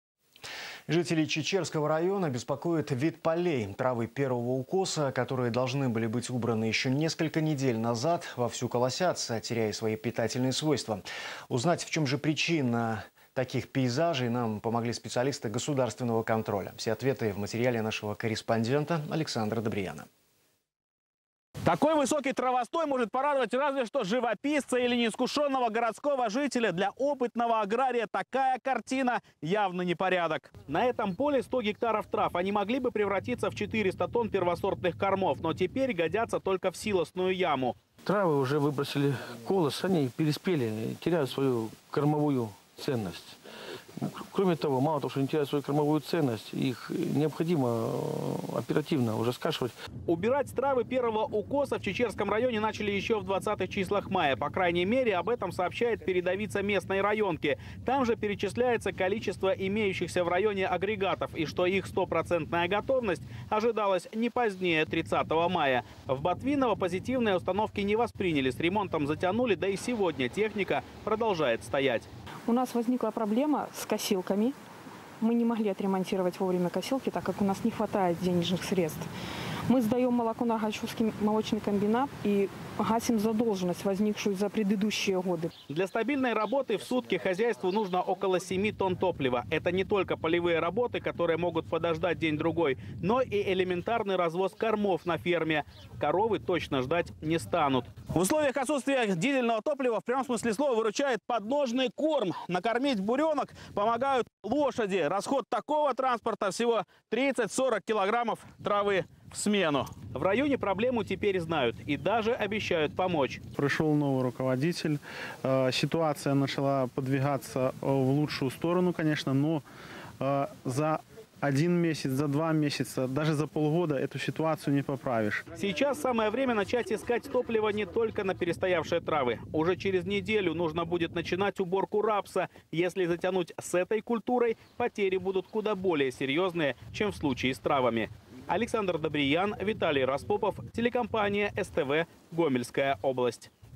Почему в Чечерском районе пропадает корм для скота
Мы не могли отремонтировать косилки, не хватает денег: почему в Чечерском районе пропадает корм для скота